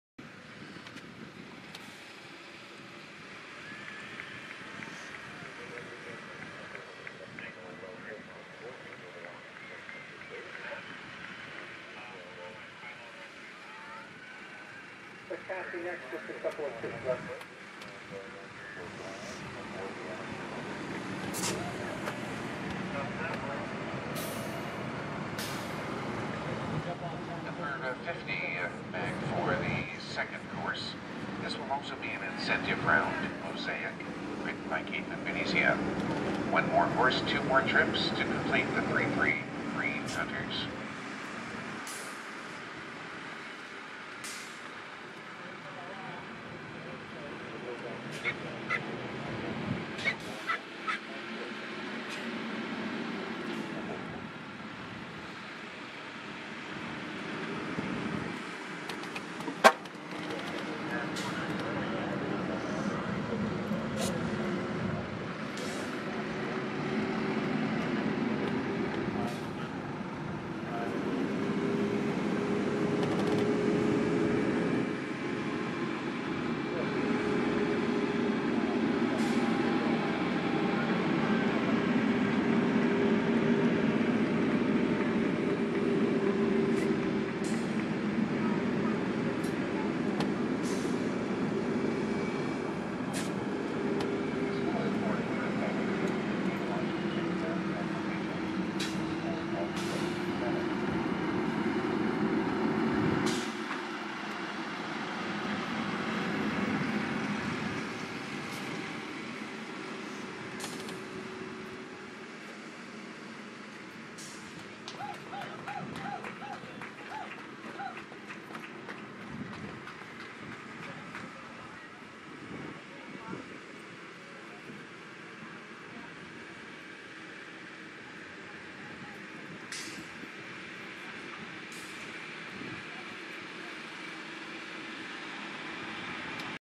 Video of MOSAIC ridden by JEANINE CASH from ShowNet!
MOSAIC ridden by JEANINE CASH at I LOVE NY HORSE SHOW 2019.  This round is from class 2020, Green Hunter 3'3 O/F,, and finished 1 of 9.